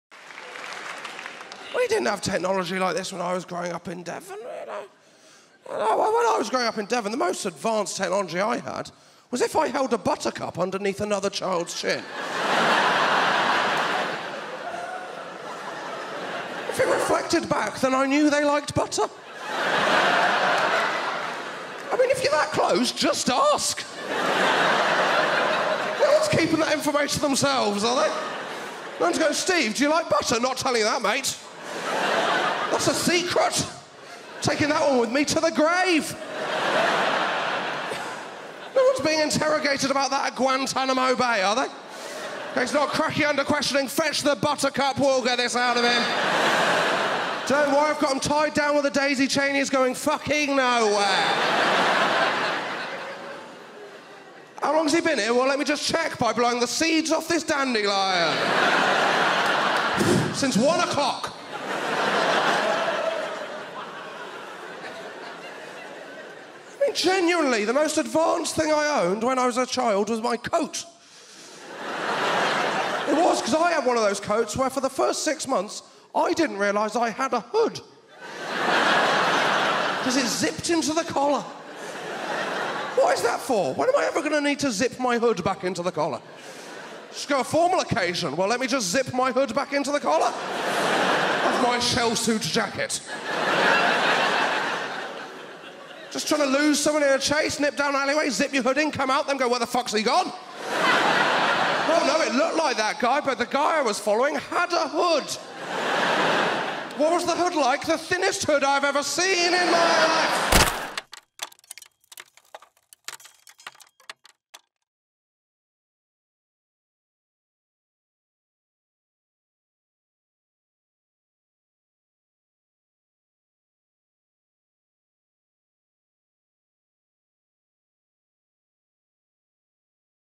My Most Technologically Advanced Coat | Josh Widdicombe | Universal Comedy
Josh Widdicombe really didn't have much technology available growing up...